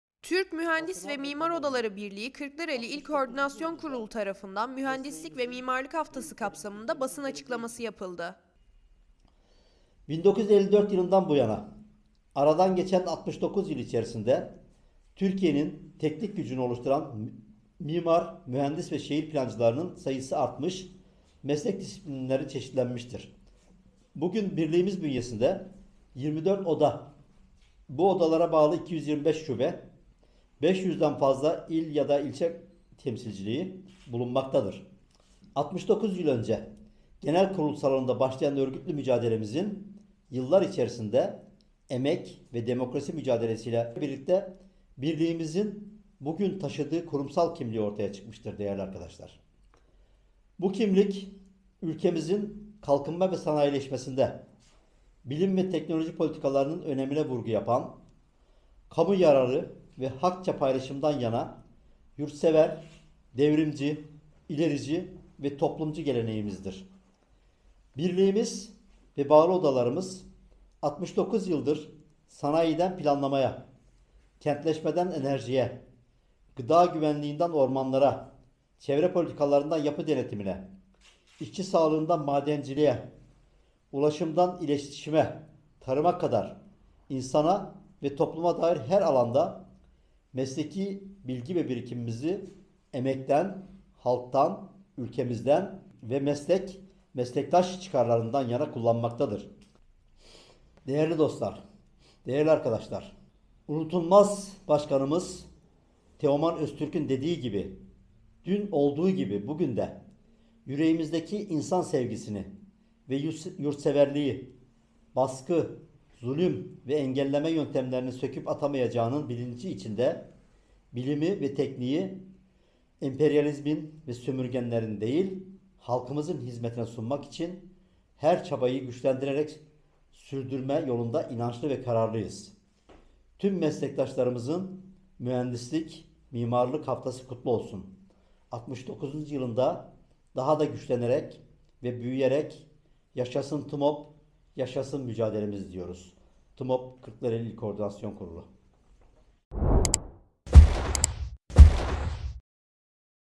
TMMOB Mühendislik ve Mimarlık Haftasına Dikkat Çekti!
Türk Mühendis ve Mimar Odaları Birliği Kırklareli İl Koordinasyon Kurulu tarafından Mühendislik ve Mimarlık Haftası kapsamında basın açıklaması yapıldı.

Güncel Haberlerimiz İçin İnternet Sitemizi Takip Edebilirsiniz...